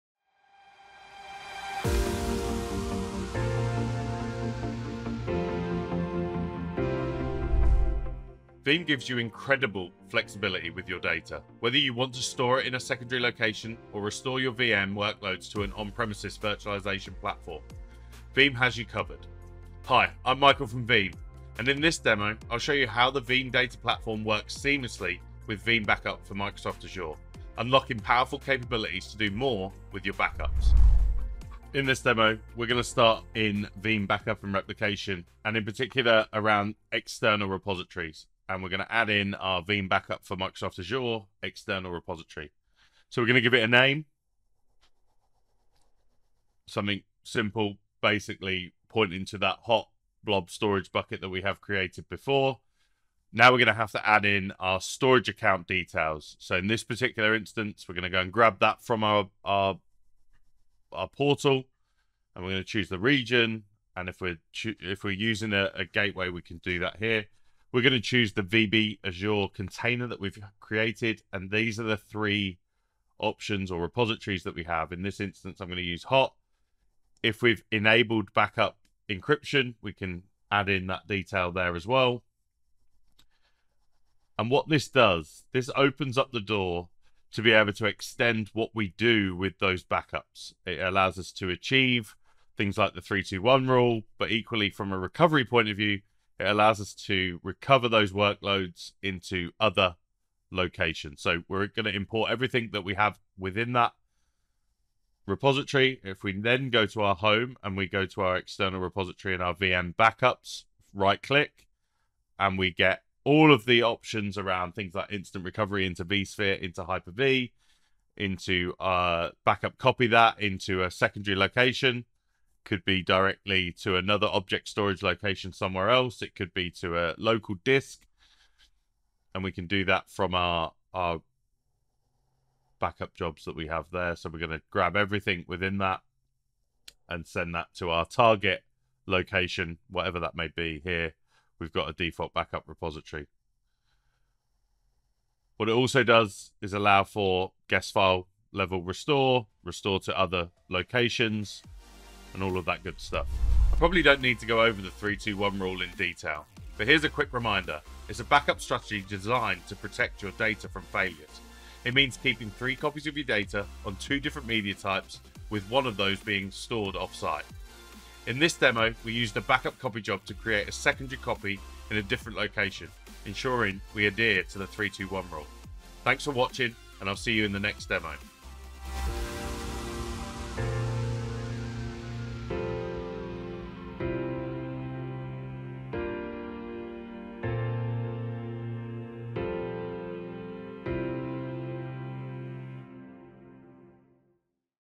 Veeam Backup for Microsoft Azure: Veeam Data Platform, External Repository, Backup Copy & Restore
Explore the versatility of your data with Veeam's robust solutions. Whether it's storing your data in a secondary location or restoring VM workloads to an on-premises platform, Veeam Backup for Microsoft Azure enables you to do more. This demo highlights the integration of Veeam Data Platform with Azure backups, showcasing how to enhance data protection strategies. We demonstrate the backup copy job functionality, ensuring compliance with the 3-2-1 rule: 3 copies of your data, 2 different media types, and 1 off-site. Strengthen your data environment and prepare for any potential failures by leveraging Veeam's innovative capabilities.

Visit Veeam.com to learn more about Veeam Backup for Microsoft Azure, part of Veeam Data Platform